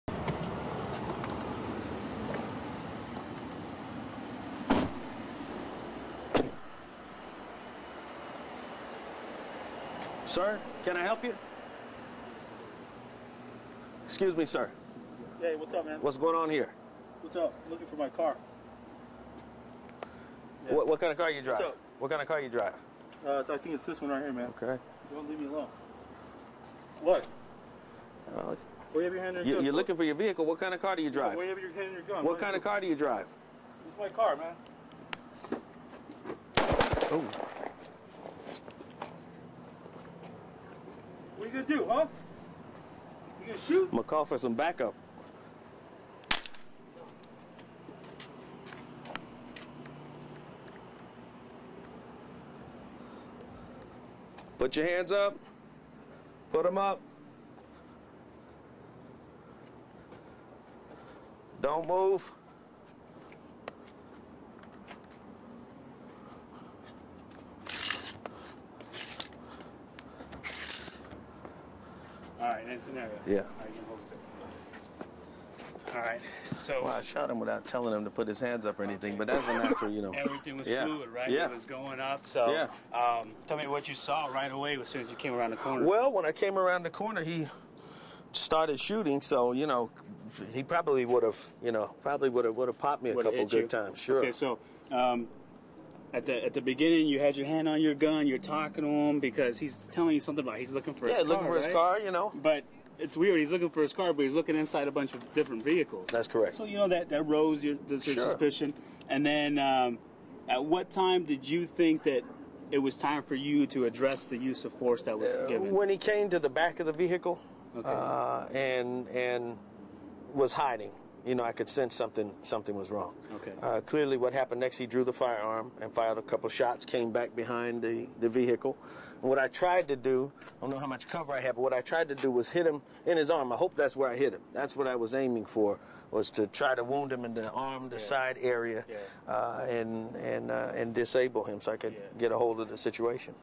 Raw video: Protester confronts suspect casing cars in police simulation (2 of 3) | FOX 10 News
Reverend Jarett Maupin, who led protests against police shooting of an unarmed man in Phoenix, participated in a use of force simulation where he had to assess a situation and react. This is the raw video from a GoPro video attached to Maupin's vest.

In this scenario Maupin is an officer responding by himself to reports of a suspect casing cars in a parking lot. It had a very surprise ending for the police officer and shows how fast things can go bad.